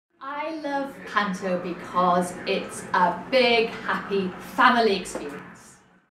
Melanie Walters - I love panto because...
Our sparkling sweet Forest Fairy Melanie Walters chats to us on PantoDay and tells us why she loves it!